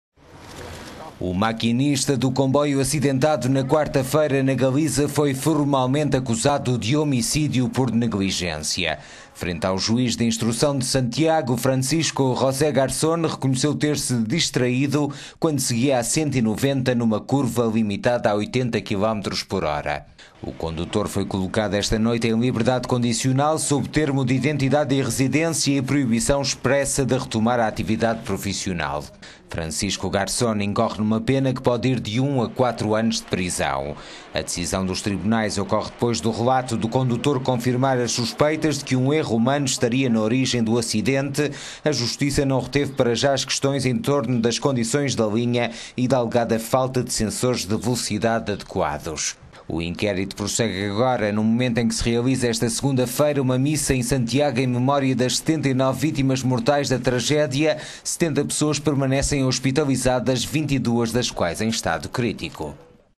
Acidente Galiza: maquinista admite excesso de velocidade "por distração"
O maquinista do comboio acidentado na quarta-feira, na Galiza, foi formalmente acusado de...

O maquinista do comboio acidentado na quarta-feira, na Galiza, foi formalmente acusado de homicídio por negligência.

Frente ao juiz de instrução de Santiago, Francisco José Garzón reconheceu "ter-se distraído" quando seguia a 190 numa curva limitada a 80km/h.

O condutor foi colocado, esta noite, em liberdade condicional, sob termo de identidade e residência e proibição expressa de retomar a atividade profissional.

Francisco Garzón incorre numa pena que pode ir de 1 a 4 anos de prisão.

A decisão dos tribunais ocorre depois do relato do condutor confirmar as suspeitas de que um erro humano estaria na origem do acidente que provocou 79 mortos e centenas de feridos.

A justiça não reteve para já as questões em torno das condições da linha e da alegada falta de sensores de velocidade adequados.

O inquérito prossegue agora num momento em que se realiza, esta segunda-feira, uma missa em Santiago, em memória das 79 vítimas mortais da tragédia.